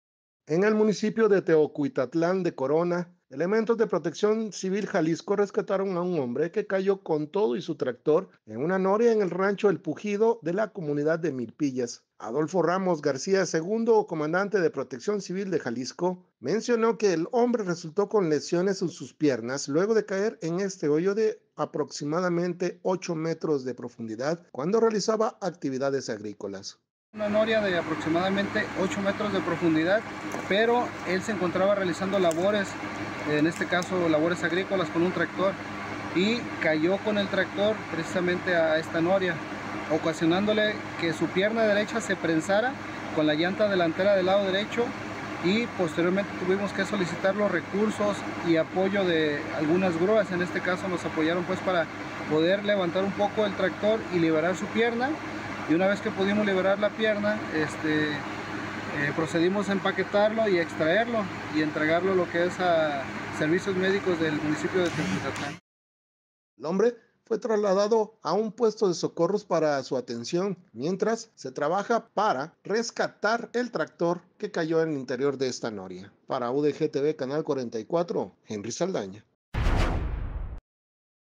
Rescatan a un hombre que cayó con todo y tractor a una Noria de 8 metros de profundidad
En el municipio de Teocuitatlán de Corona, elementos de Protección Civil Jalisco rescataron a un hombre que cayó con todo y su tractor a una Noria en el rancho El Pujido de la comunidad de Milpilla.